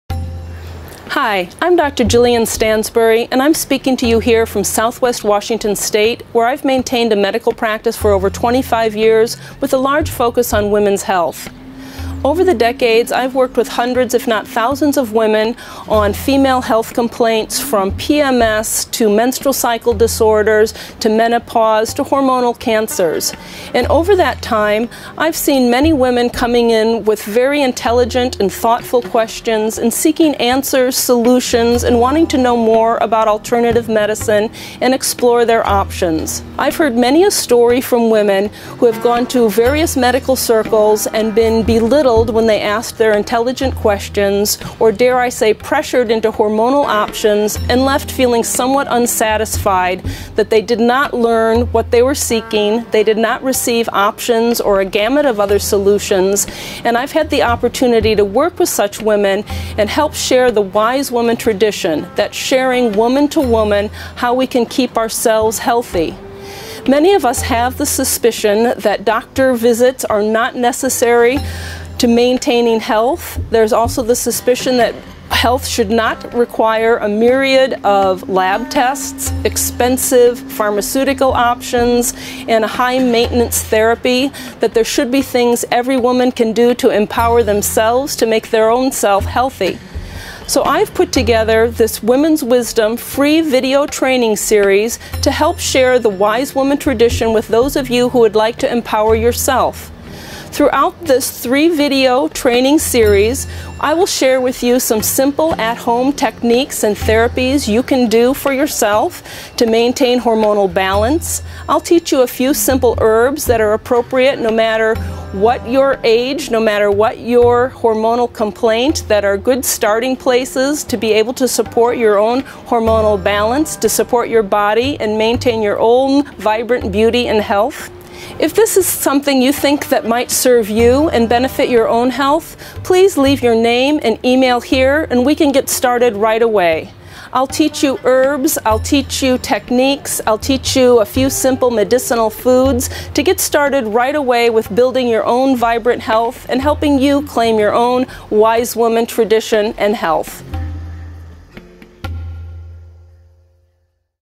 Dr Jillian Stansbury 438221046
This short  video is the intro to a larger course produced with Floracopeia, and focuses on empower and inspiring  women to enjoy better health and hormonal balance using herbs and natural medicines.